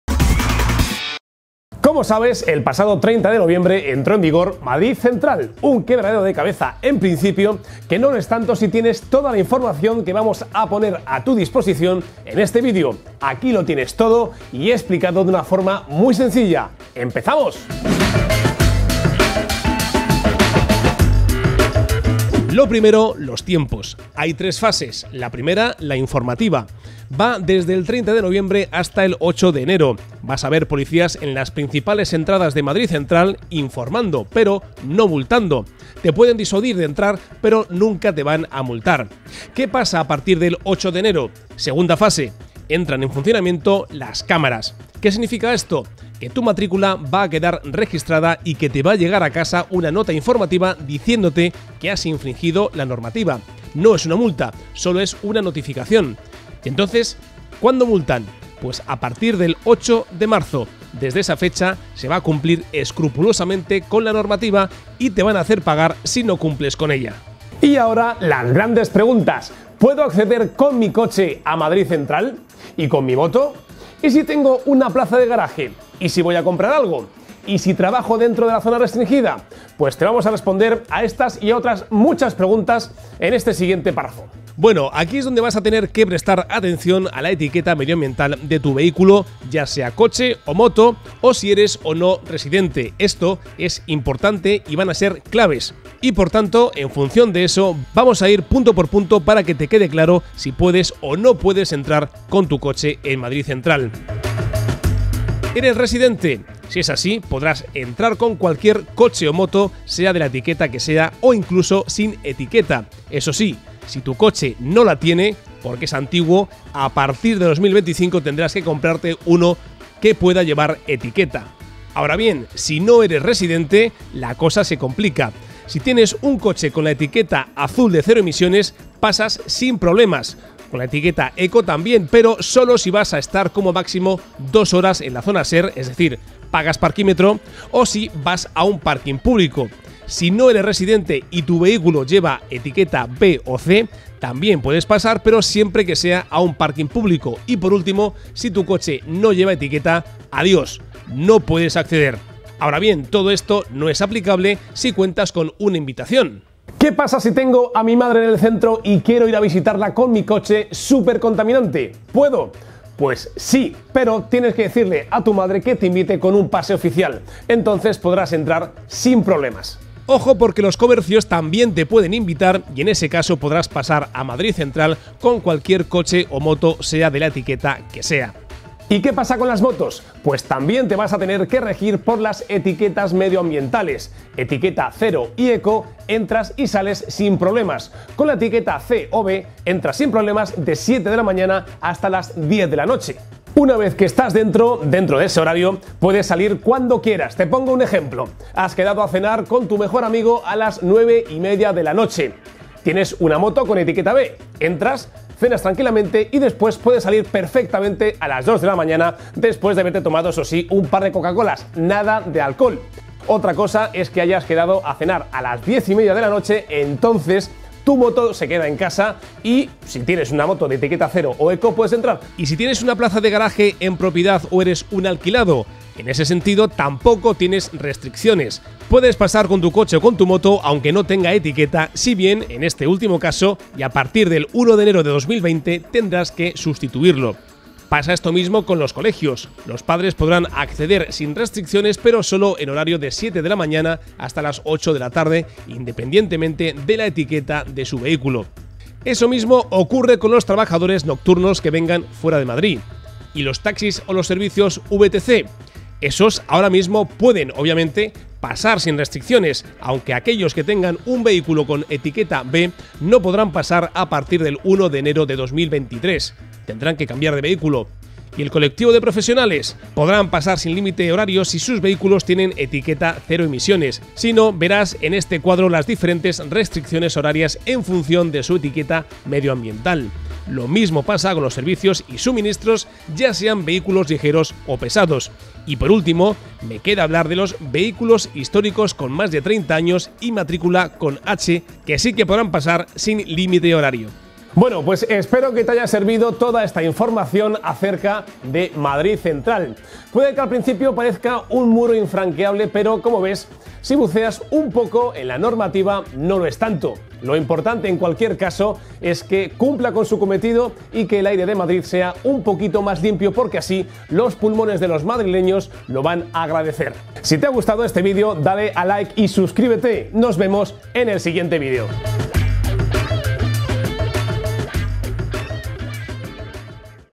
Madrid Central, ¿puedes entrar con tu coche? Te lo decimos
Si andas a vueltas con el nuevo plan del consistorio madrileño de Madrid Central, aquí tienes las líneas maestras. En resumidas cuentas, pueden entrar:

- Los residentes empadronados en los barrios del distrito Centro podrán acceder a Madrid Central con cualquier vehículo siempre que lo tengan en propiedad o en usufructo.
- Los vehículos con etiqueta ECO o CERO emisiones también podrán circular sin problemas por Madrid Central.
- Los vehículos con etiqueta B o C podrán pasar a Madrid Central, siempre que estacionen en un parking público o privado.
- Los invitados por residentes empadronados podrán pasar a Madrid Central (hasta 2020, con cualquier vehículo; a partir de esta fecha, solo con aquellos que tengan etiqueta medioambiental).
- Los propietarios de una plaza de garaje en la zona acotada como Madrid Central podrán entrar para dejar el coche en esa plaza. (A partir de 2020, necesitarán tener etiqueta medioambiental).
- Coches de carsharing: siguen la norma general (han de tener la etiqueta de la DGT).
- Quienes tengan un establecimiento ubicado en Madrid Central deberán cumplir la normal general en función del tipo de vehículo que lleven. Los concesionarios y talleres tendrán un tratamiento especial.
- Los taxis y vehículos con licencia VTC no podrán acceder al área de Madrid Central sin etiqueta.
- Las personas con movilidad reducida podrán entrar a la zona de Madrid Central con cualquier vehículo (a partir de 2025, solo podrán entrar con aquellos sin etiqueta si son vehículos adaptados).
- Los vehículos de empresas y profesionales de servicios y suministros tienen un horario determinado para acceder a Madrid Central.